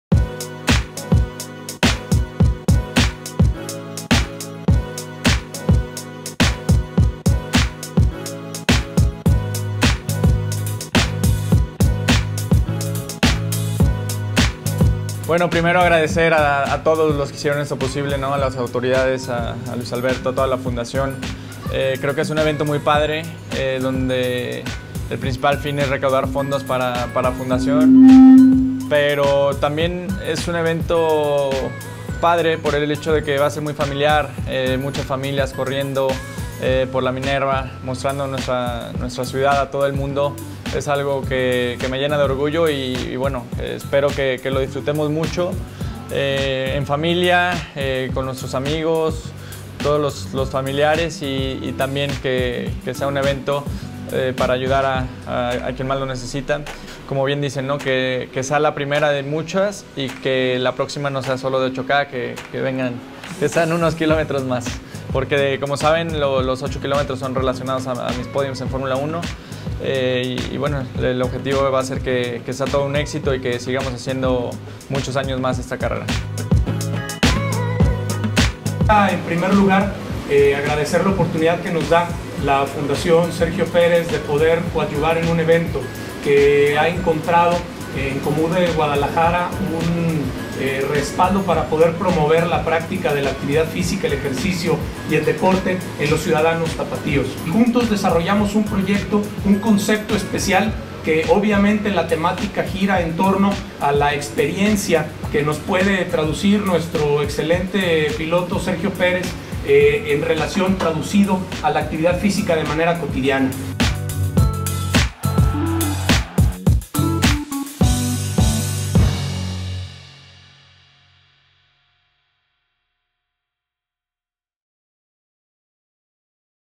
ResponsabilidadSocial, presenta Sergio Pérez su carrera 8K Checo Run l
El piloto mexicano de la escudería de Fórmula 1 SportPesa Racing Point presentó la carrera 8K Checo Run en beneficio de su fundación para niños de escasos recursos.